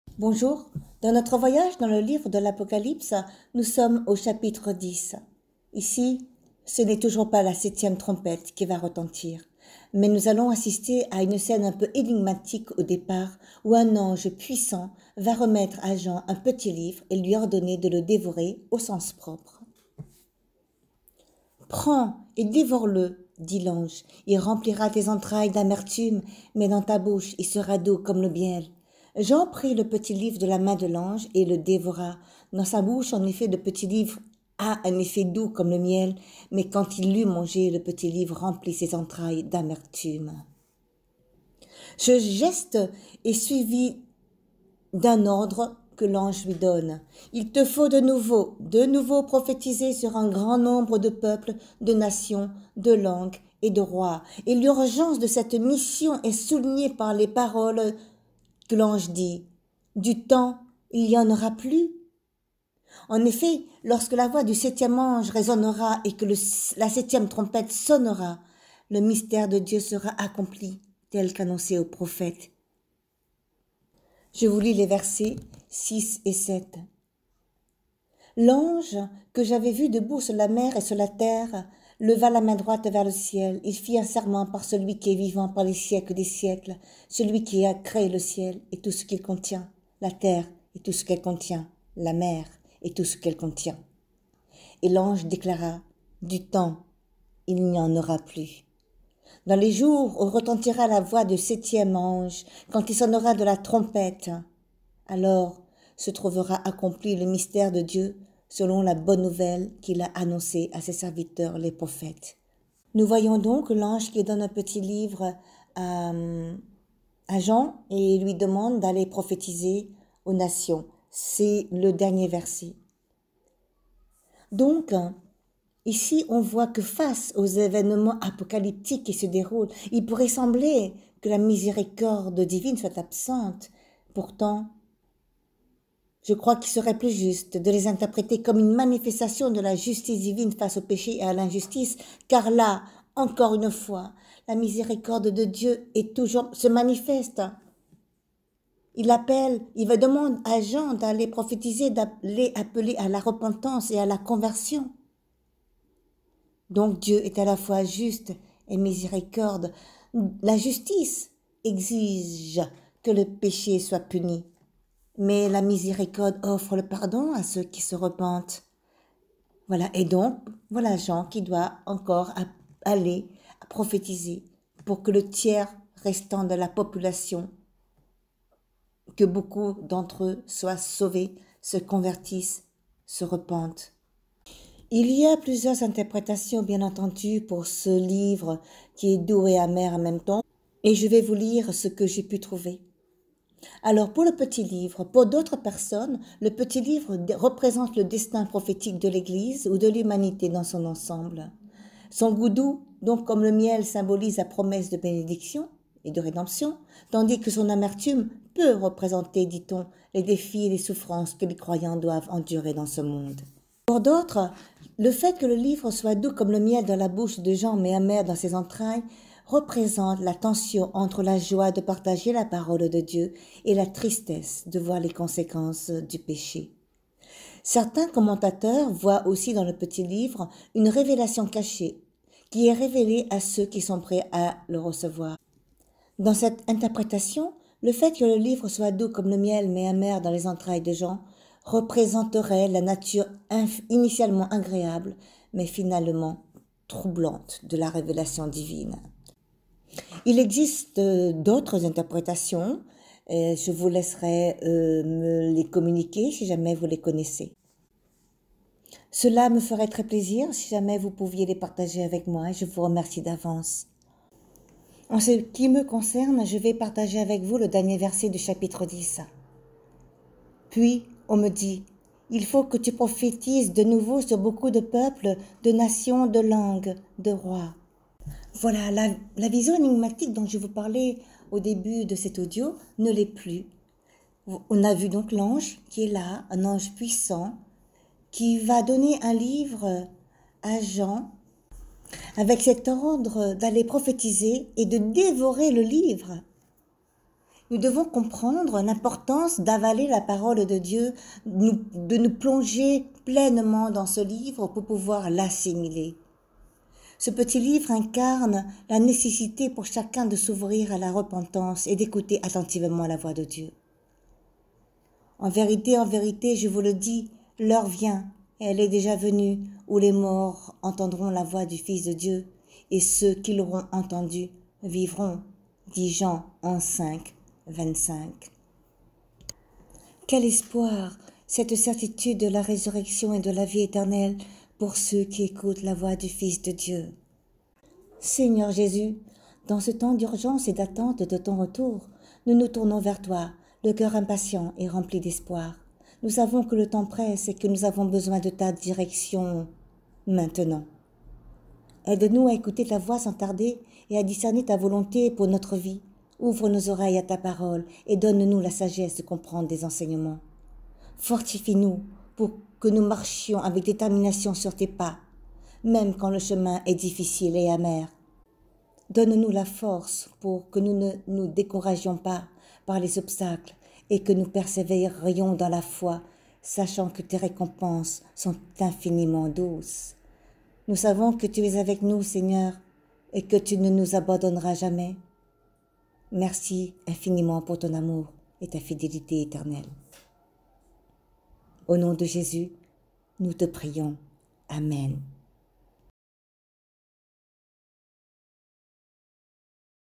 APOCALYPSE 10 : URGENCE DE LA PROPHÉTIE - L'ANGE REMET LE PETIT LIVRE À JEAN
APOCALYPSE 10  - 01 Et j’ai vu un autre ange, plein de force, descendre du ciel, ayant une nuée pour manteau, et sur la tête un halo de lumière ; son visage était comme le soleil, et ses jambes comme des colonnes de feu.
02 Il tenait à la main un PETIT LIVRE ouvert. Il posa le pied droit sur la mer, et le gauche sur la terre ;
03 il cria d’une voix forte, comme un lion qui rugit. Et quand il cria, les sept tonnerres parlèrent, faisant résonner leur voix.
04 Et quand les sept tonnerres eurent parlé, j’allais me mettre à écrire ; mais j’entendis une voix venant du ciel qui disait : « Ce que viennent de dire les sept tonnerres, garde-le scellé, ne l’écris pas ! »
05 Et l’ange que j’avais vu debout sur la mer et sur la terre leva la main droite vers le ciel ;
06 il fit un serment par celui qui est vivant pour les siècles des siècles, celui qui a créé le ciel et tout ce qu’il contient, la terre et tout ce qu’elle contient, la mer et tout ce qu’elle contient. Il déclara : « DU TEMPS, IL N'Y EN AURA PLUS !
07 Dans les jours où retentira la voix du septième ange, quand il sonnera de la trompette, alors se trouvera accompli le mystère de Dieu, selon la bonne nouvelle qu’il a annoncée à ses serviteurs les prophètes. »
08 Et la voix que j’avais entendue, venant du ciel, me parla de nouveau et me dit : « Va prendre le livre ouvert dans la main de l’ange qui se tient debout sur la mer et sur la terre. »
09 JE M’AVANÇAI VERS L’ANGE POUR LUI DEMANDER DE ME DONNER LE PETIT LIVRE. IL ME DIT : « PRENDS, ET DÉVORE-LE ; IL REMPLIRA TES ENTRAILLES D’AMERTUME, MAIS DANS TA BOUCHE IL SERA DOUX COMME LE MIEL.
10 Je pris le petit livre de la main de l’ange, et je le dévorai. Dans ma bouche il était doux comme le miel, mais, quand je l’eus mangé, il remplit mes entrailles d’amertume.
11 ALORS ON ME DIT : « IL TE FAUT DE NOUVEAU PROPHÉTISER SUR UN GRAND NOMBRE DE PEUPLES, DE NATIONS, DE LANGUES ET DE ROIS. »

 Jean 5:25 : En vérité, en vérité, je vous le dis, l'heure vient, et elle est déjà venue, où les morts entendront la voix du Fils de Dieu; et ceux qui l'auront entendue vivront.

apocalypse10